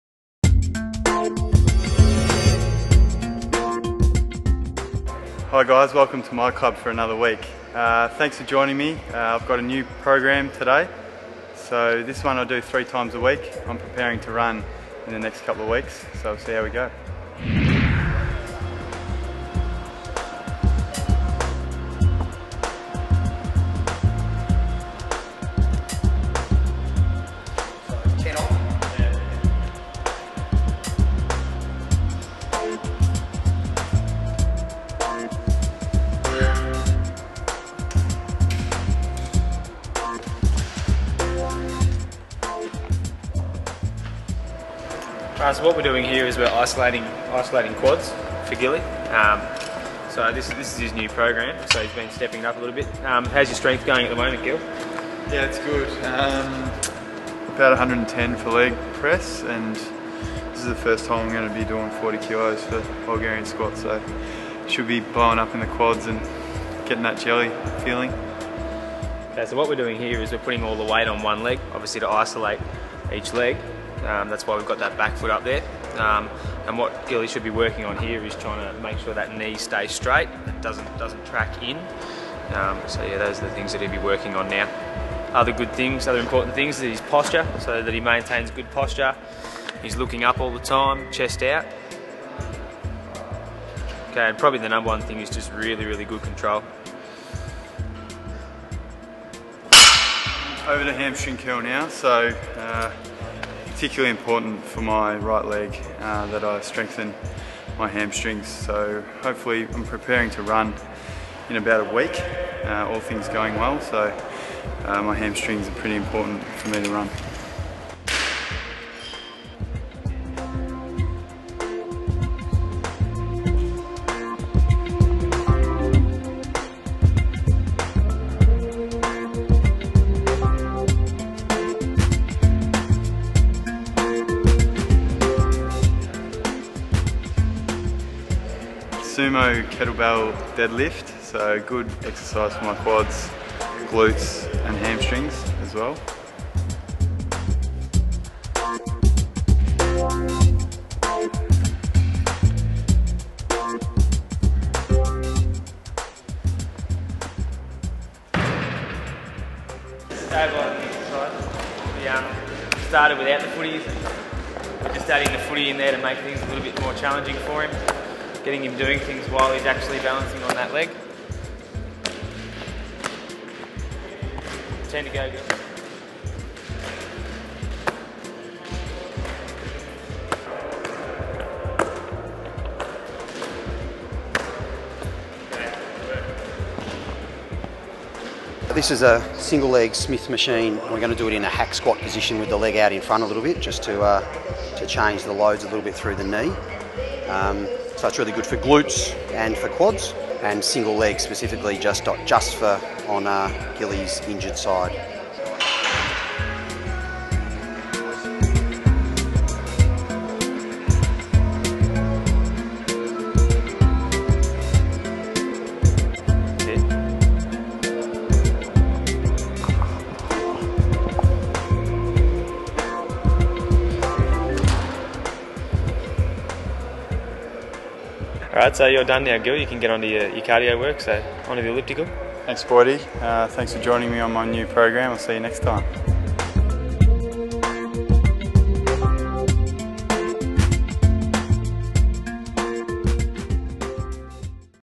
HawksTV My Club: Gilham's new rehab program
Stephen Gilham prepares to run with a new rehab program.